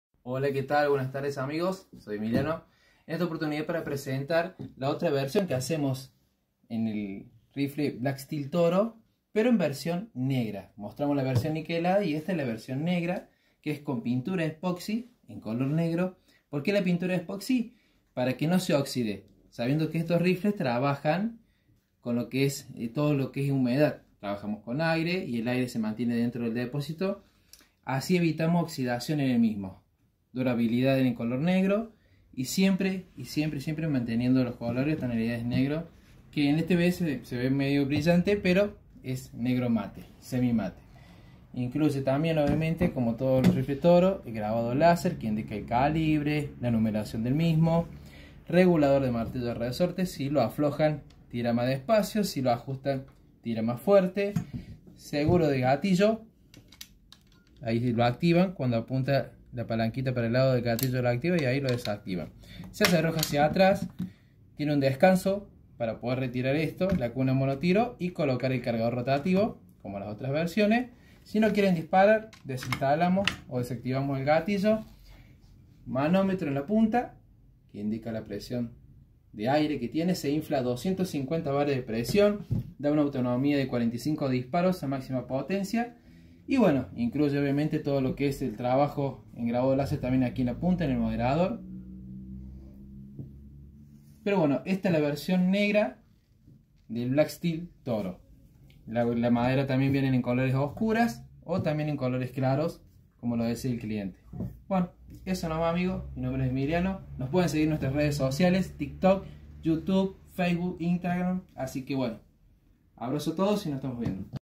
BLACK STEEL TORO
Especificaciones Técnicas:
MODELO TORO
CALIBRE 6.35 mm
TIPO DE INFLADO PCP
VELOCIDAD 950 FPS con 25gr
AUTONOMÍA 35 disparos
TAMAÑO DEL DEPOSITO 350cc
LARGO DEL CAÑON 57cm
LARGO TOTAL 1,10 mts
PESO  3.8KG
CULATA  Madera
MIRA ABIERTA No
MULTI-SHOT Si
RIEL PARA MIRA / TIPO  Si / 11mm.
RIEL PARA ACCESORIOS No.
REGULADOR DE VÁLVULA No
REGULADOR DE POTENCIA Si
GATILLO REGULABLE No
SISTEMA DE CARGA Cerrojo
PRESIÓN DE INFLADO MÁXIMA 250 bar.
MANÓMETRO Si
ACCESORIOS INCLUIDOS Bullbarrel incorporado
Cargador 9 Disparos
Acople de Carga
Kit de O’rings